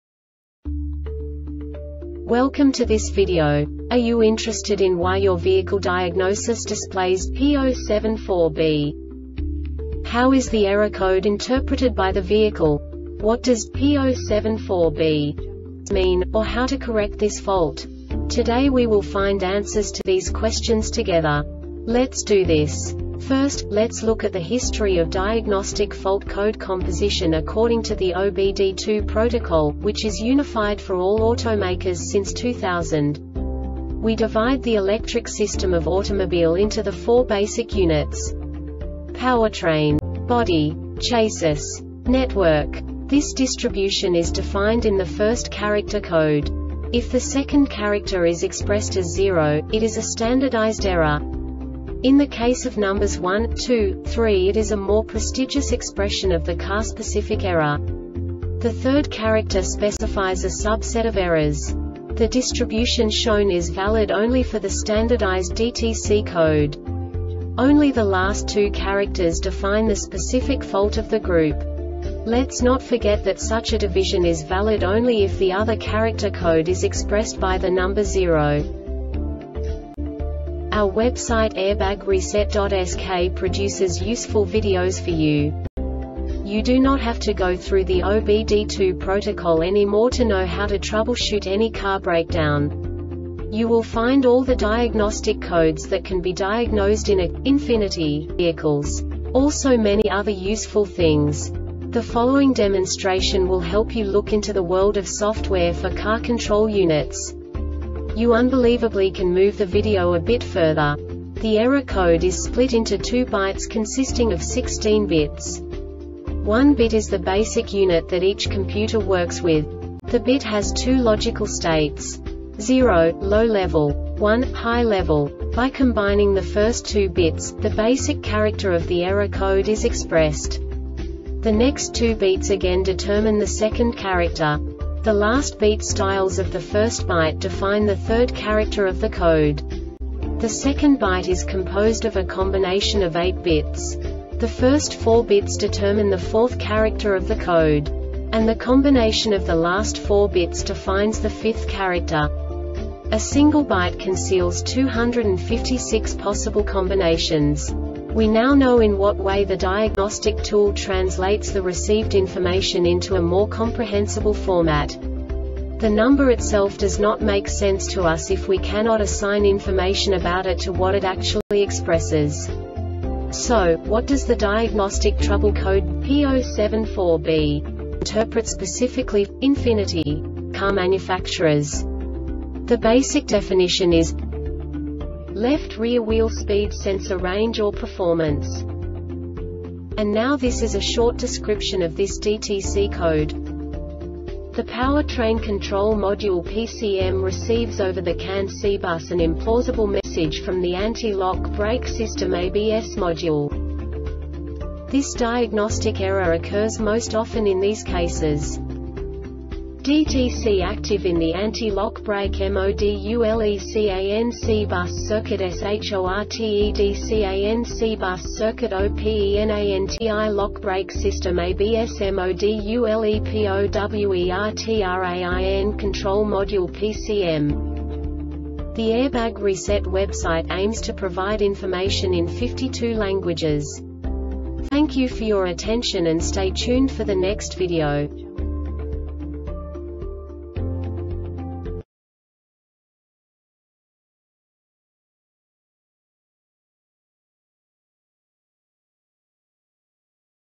DTC Ford P074B Short Explanation
Contents:
0:21 Basic DTC analysis according to OBD2 protocol standard.
1:48 Insight into programming
2:58 A diagnostically understandable description of the Diagnostic Trouble Code
3:05 Basic error definition

Make:
Ford
Code:
P074B
Definition:
Left Rear Wheel Speed Sensor Range or Performance
Description:
Cause:
DTC ACTIVE IN THE ANTI-LOCK BRAKE MODULECAN C BUS CIRCUIT SHORTEDCAN C BUS CIRCUIT OPENANTI-LOCK BRAKE SYSTEM (ABS) MODULEPOWERTRAIN CONTROL MODULE (PCM)